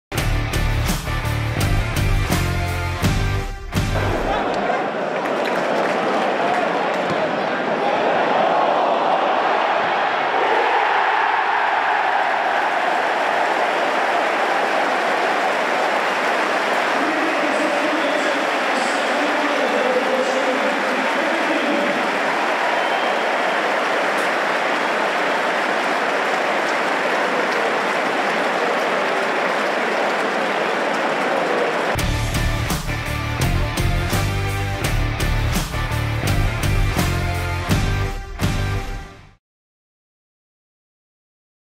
GoalCam: Gary Madine's second
Through the net footage of Gary Madine completing his brace